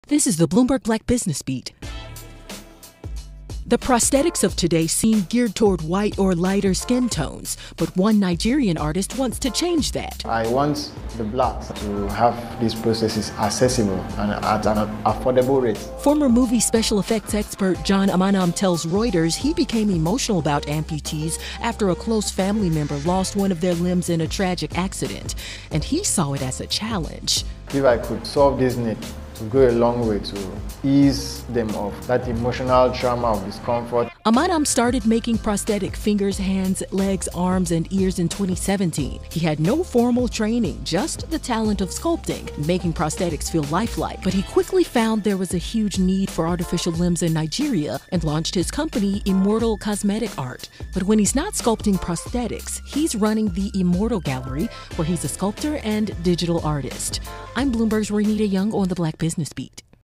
Nigerian Sculptor Creates Lifelike Prosthetics for People of Color
Today on the BloombergBlackBusinessBeat: meet Sculptor John Amanam, a former special effects artist who now creates lifelike prosthetics for people of color. More in the clip!

Photo courtesy of the Immortal Gallery.